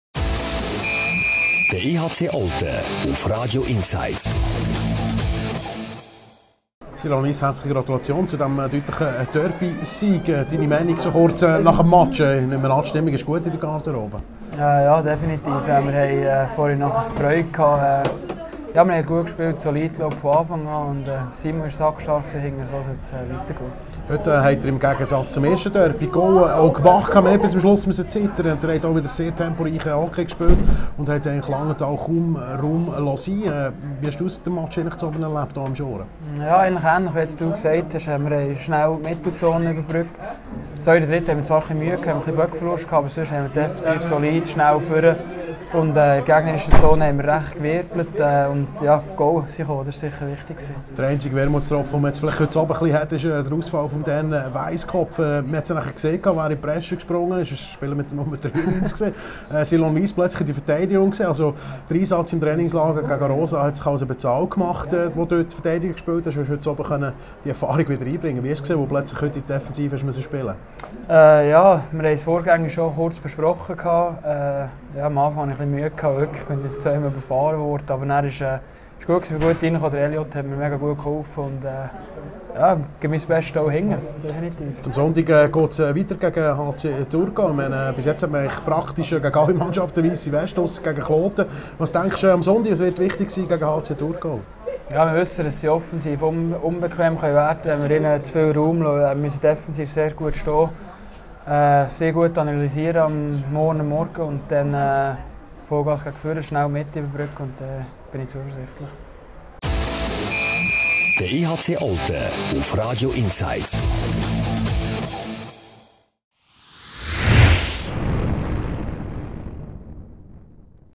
Derbysieg für den EHC Olten
Der EHC Olten schlägt den Derbyrivalen aus Langenthal diskussionslos mit 5:0 und bleibt im Schoren auch im zweiten Spiel ohne Gegentor.